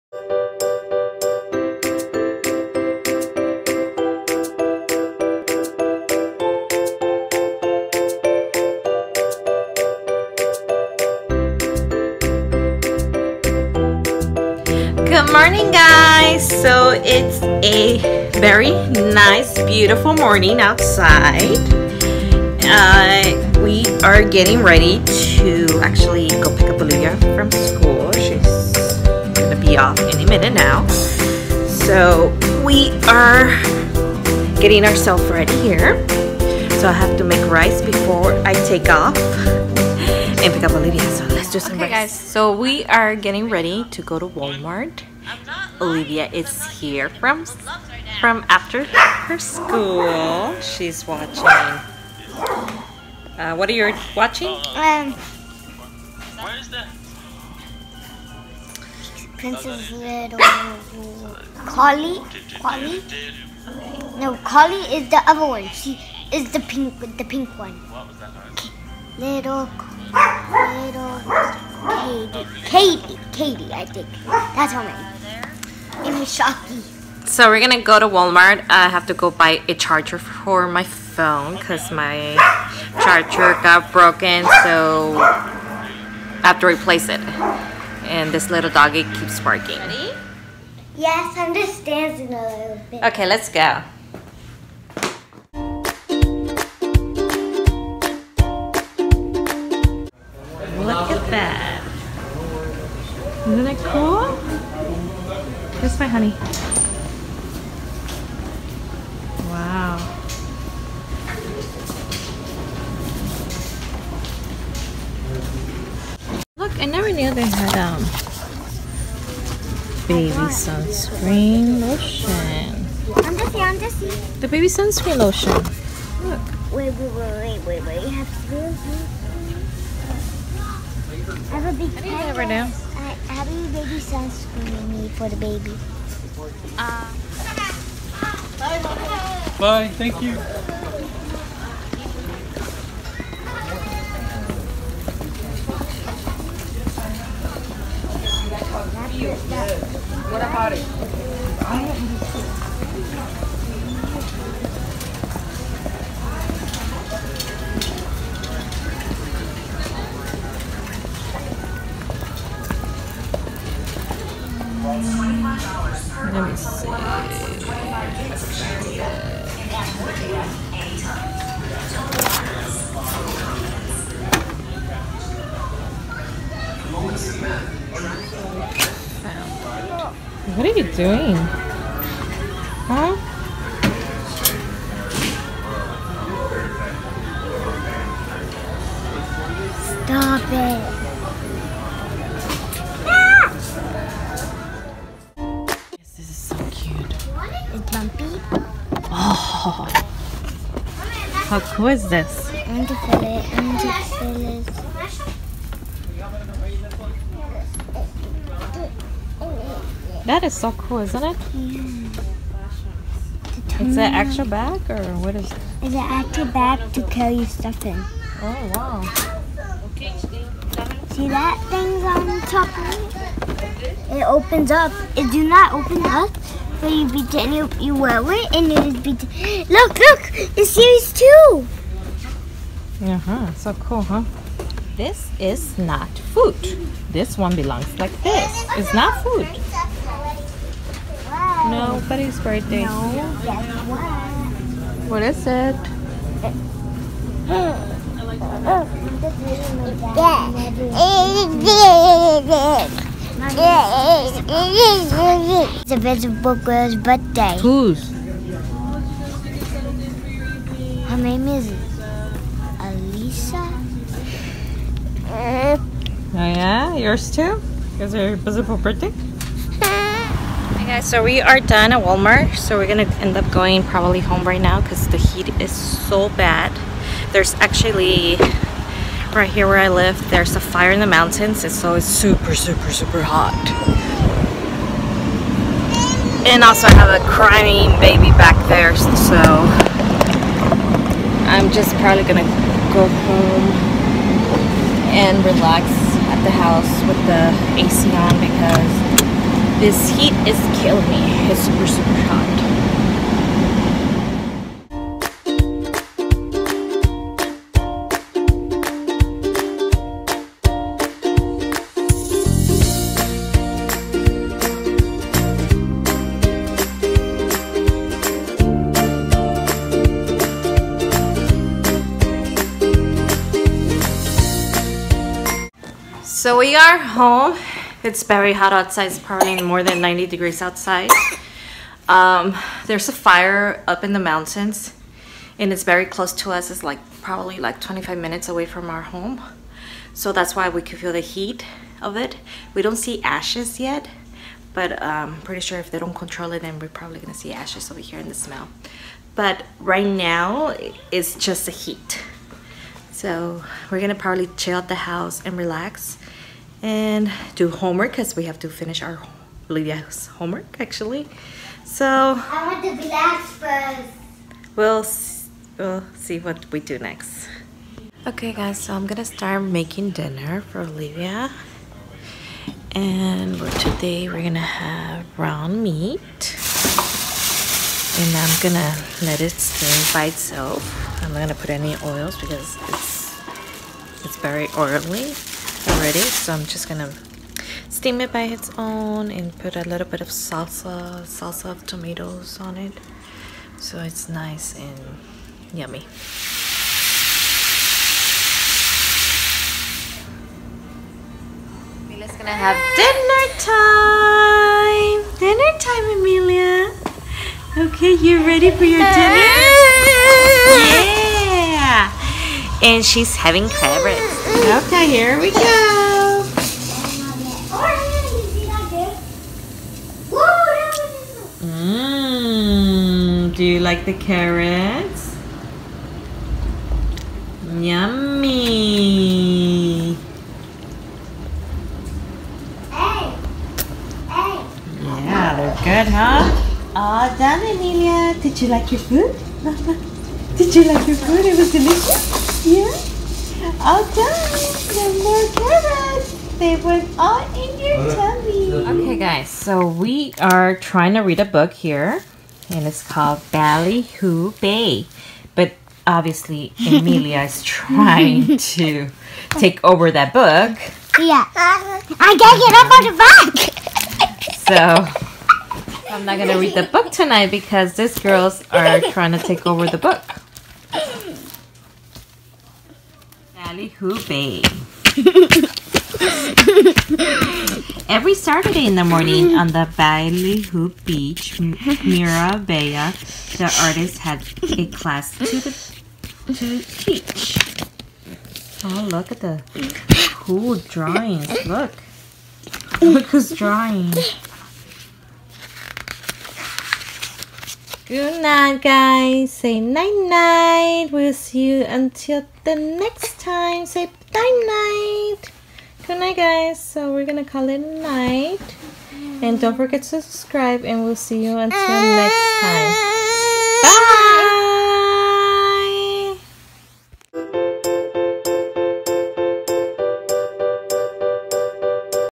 Fire Up In The Mountains
Fire up in the Mountains Close to our Home (25 Min way ) from the fire you can feel the heat it was around 99 fahrenheit.